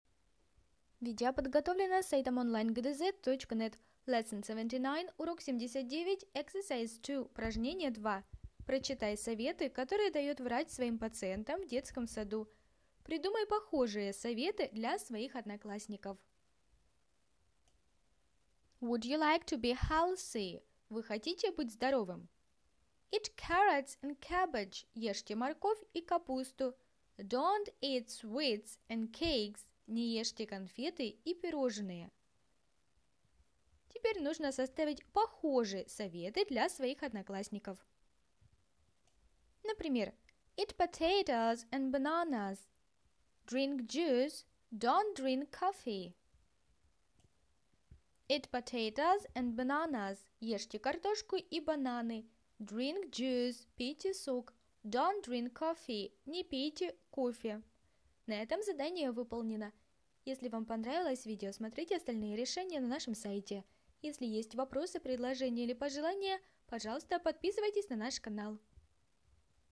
Урок 79 Задание №2 - Английский язык "Enjoy English" 1 класс (Биболетова)
Чтобы выполнить упражнение 2 (урок 79), необходимо прочитать советы, которые дает врач своим пациентам в детском саду; придумать похожие советы для своих одноклассников. Без труда с этим справиться поможет наше видео.